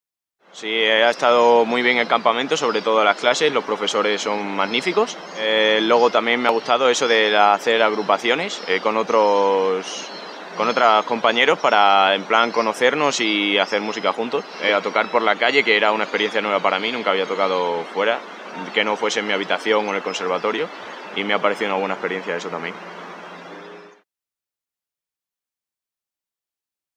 Testimonios alumnos Verano Musical
Vídeos donde alumnos del curso Verano Musical dan su opinión sobre el mismo. Verano Musical es un curso para violín, viola, violonchelo y contrabajo con profesores de talla internacional. La edición de Verano Musical 2014 se celebrará en Peñafiel del 18 al 27 de Julio del 2014.